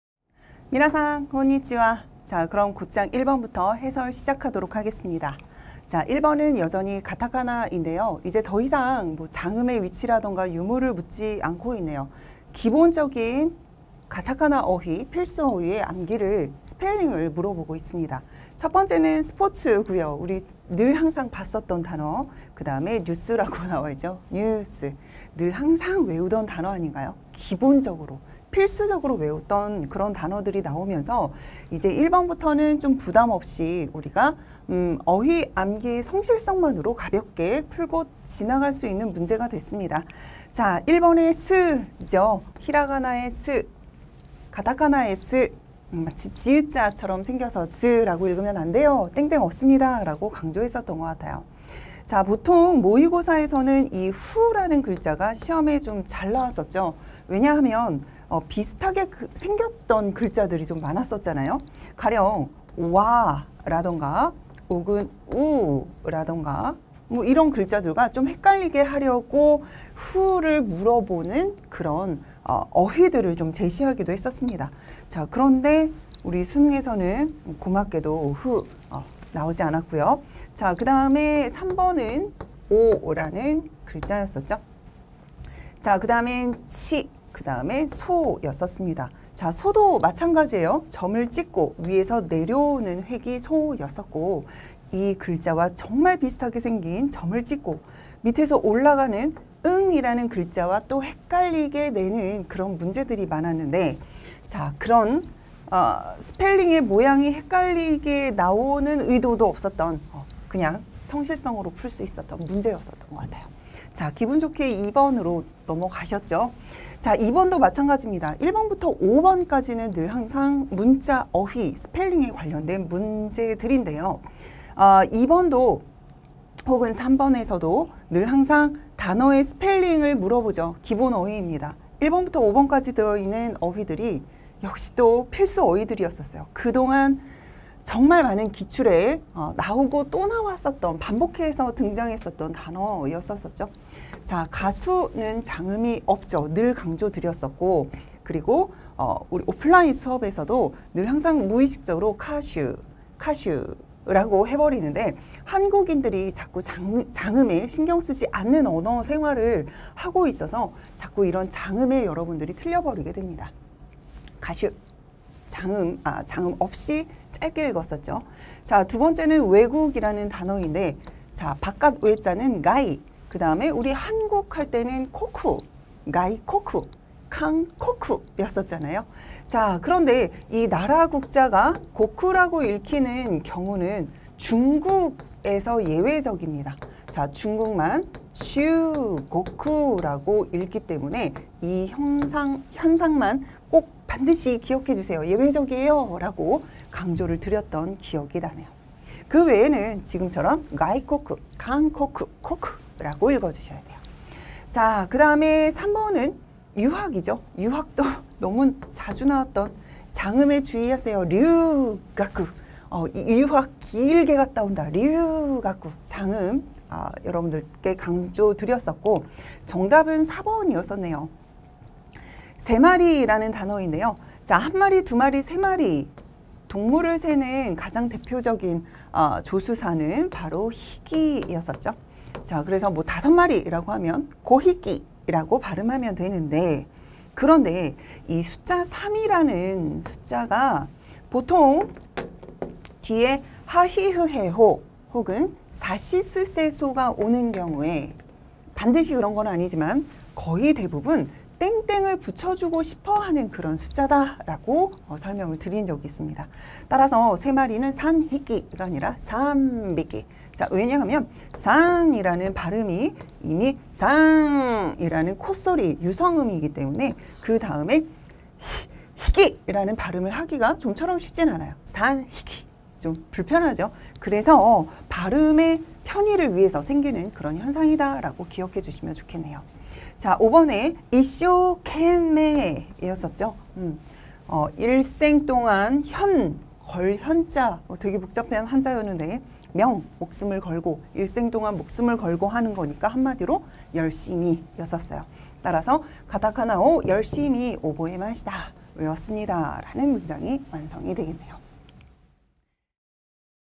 [메가스터디] 일본어 이선옥쌤 - 2023학년도 수능 일본어 해설강의
일본어 이선옥 선생님의 2023학년도 수능 일본어 해설강의 영상입니다.

- 일본어 이선옥 선생님을 더 알고싶다면?